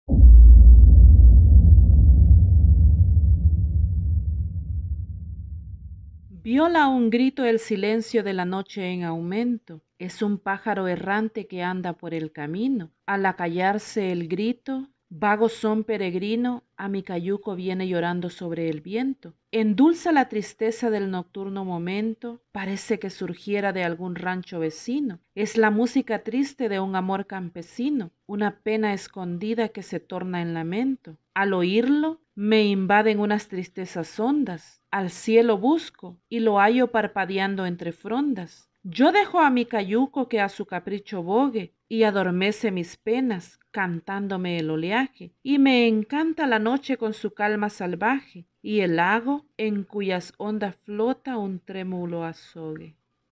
36- LUNA EN LAS ONDAS/ JICARAS TRISTES DE ALFREDO ESPINO
36- LUNA EN LAS ONDAS
Viola un grito el silencio de la noche en aumento;
es un pájaro errante que anda por el camino...
Al acallarse el grito, vago son peregrino
a mi cayuco viene llorando sobre el viento...

Endulza la tristeza del nocturnal momento;
parece que surgiera de algún rancho vecino;
es la música triste de un amor campesino,
una pena escondida que se torna en lamento...

Al oírlo, me invaden unas tristezas hondas...
Al cielo busco, y lo hallo parpadeando entre frondas...
Yo dejo a mi cayuco que a su capricho bogue:

y adormece mis penas, cantándome, el oleaje;
y me encanta la noche con su calma salvaje,
y el lago, en cuyas ondas flota un trémulo azogue...